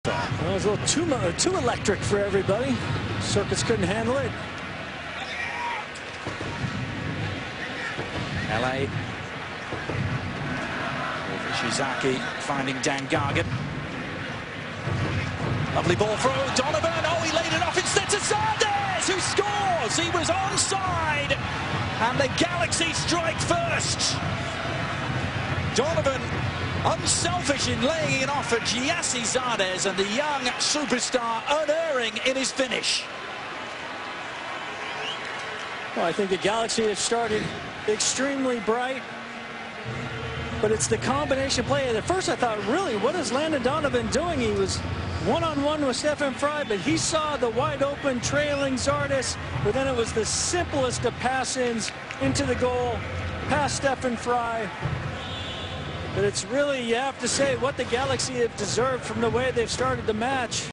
GOAL: Gyasi Zardes finishes off a Donovan set up | Seattle Sounders vs LA Galaxy
Goal!  Seattle Sounders FC 0, LA Galaxy 1. Gyasi Zardes (LA Galaxy) from the center of the box.  Assisted by Landon Donovan.